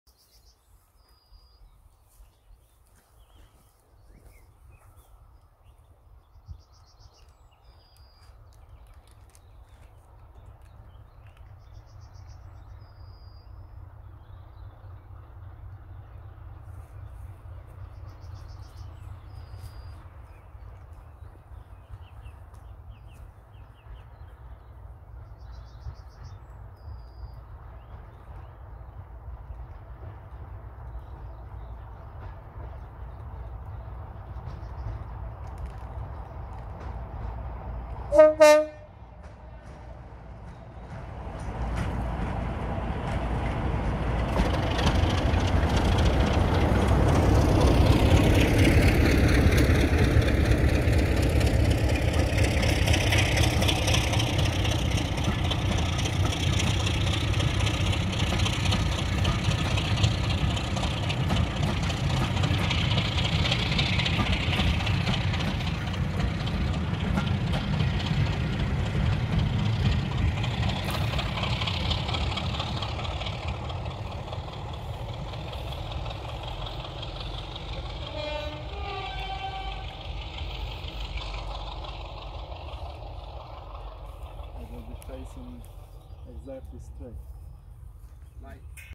33108 BEWDLEY TUNNEL 17/05/24
33108 leads the 1640 Bridgnorth-Kidderminster service.